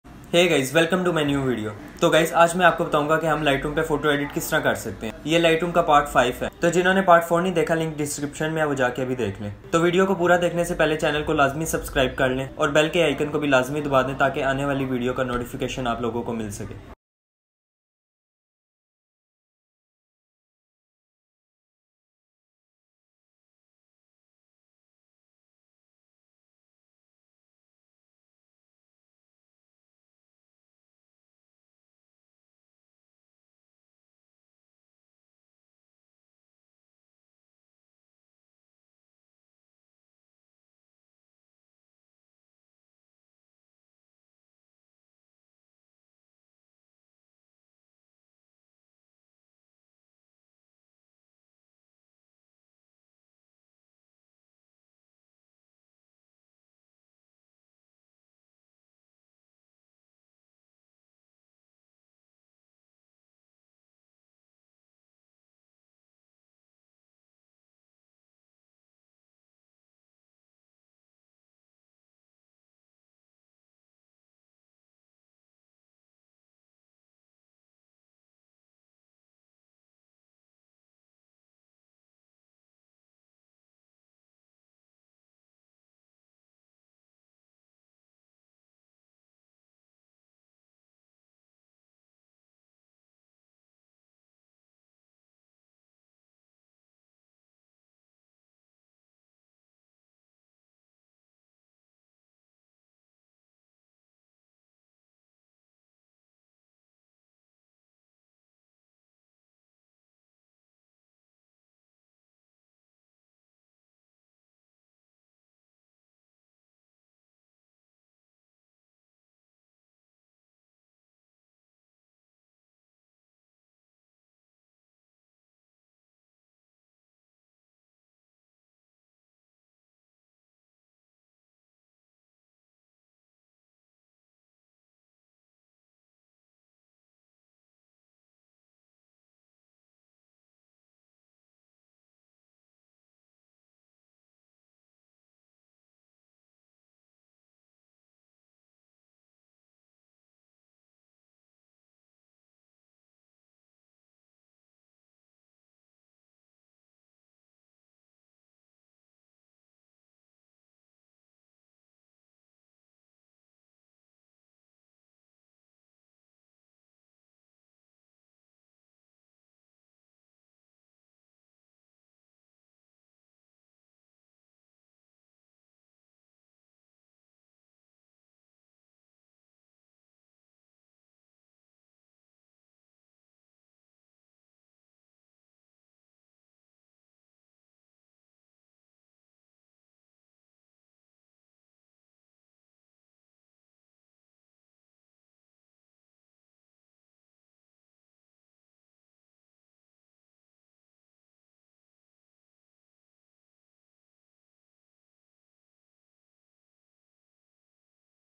How To Edit Photo In Lightroom || (Part 5) || Lightroom Photo Editing || Moeez Editing Zone
Hi Guys !
In this video, I will show you how to Edit Blackish Tone Filters using in Lightroom mobile.